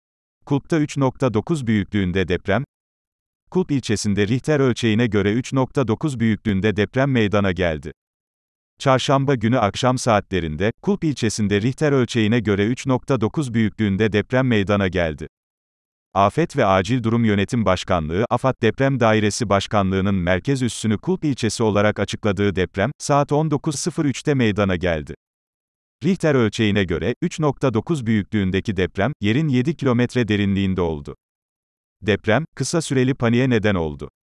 Kulp’ta 3 9 büyüklüğünde deprem
Kulp ilçesinde Richter ölçeğine göre 3.9 büyüklüğünde deprem meydana geldi.
Çarşamba günü akşam saatlerinde,Kulp ilçesinde Richter ölçeğine göre 3.9 büyüklüğünde deprem meydana geldi.
Afet ve Acil Durum Yönetim Başkanlığı (AFAD) Deprem Dairesi Başkanlığı'nın merkez üssünü Kulp ilçesi olarak açıkladığı deprem, saat 19.03'te meydana geldi. Richter ölçeğine göre, 3.9 büyüklüğündeki deprem, yerin 7 kilometre derinliğinde oldu. Deprem, kısa süreli paniğe neden oldu.